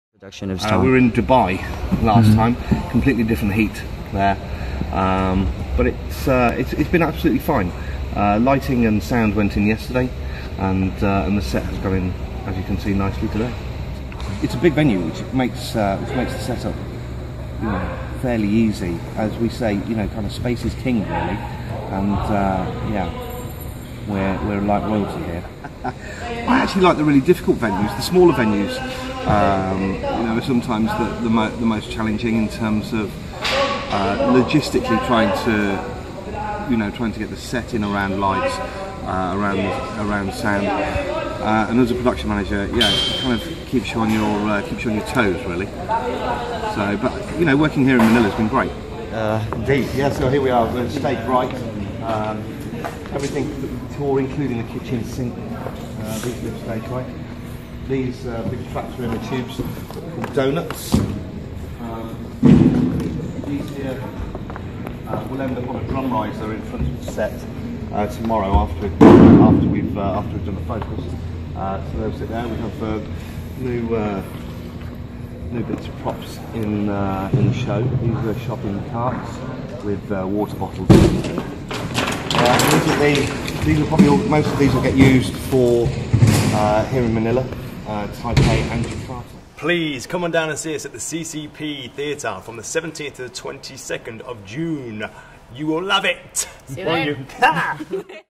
VIDEO: Exclusive STOMP Backstage Tour at the CCP
Almost everyone is abuzz with the STOMP-fever inside the Cultural Center of the Philippines' (CCP) Tanghalang Nicanor Abelardo as the main stage slowly comes together in a thick hectic air-- urbanized and industrialized with the hip backdrop and other stage production elements of STOMP--in preparation for its second Manila staging brought to the country by Concertus Manila and Lunchbox Theatrical Productions.

Importing a vast array of props and STOMP instruments to kick off STOMP's first performance--in a limited eight-day-run, which premiered last night, June 17, and runs until June 22, 2014--Production Manager Jason Culverwell sits down for a quick chat with BroadwayWorld.com to talk about the setting up and preparation of CCP's main theater stage.